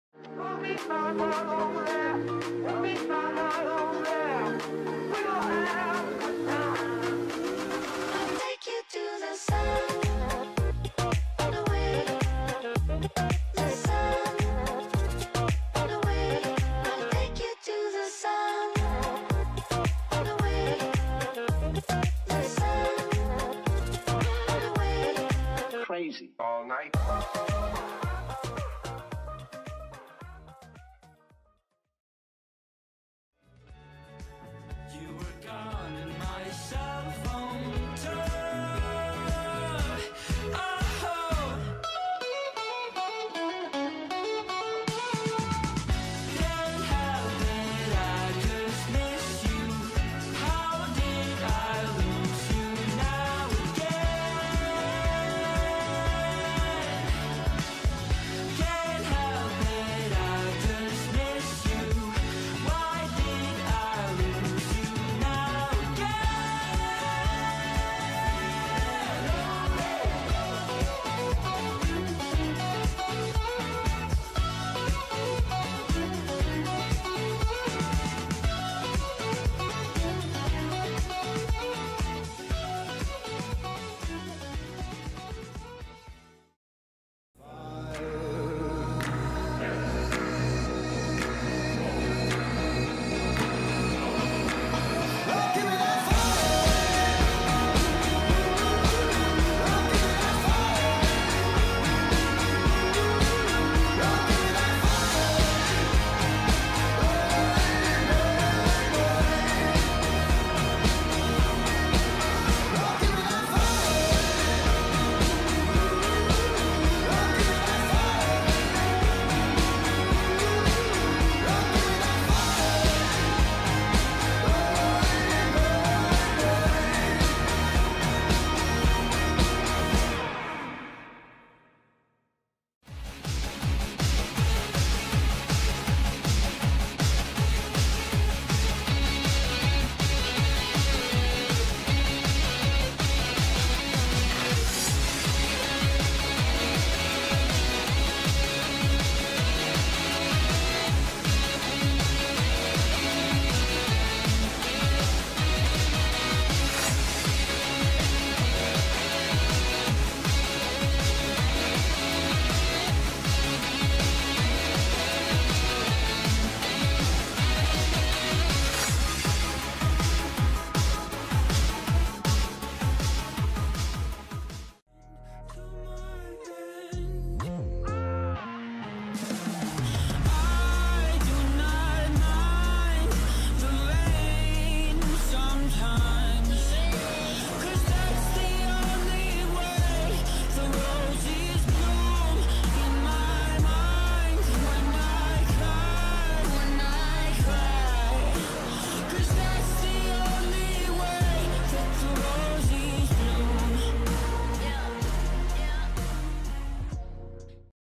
All Razlika UHC Intros! (S1-S5)
This is a compilation of all the intros for the seasons of Razlika UHC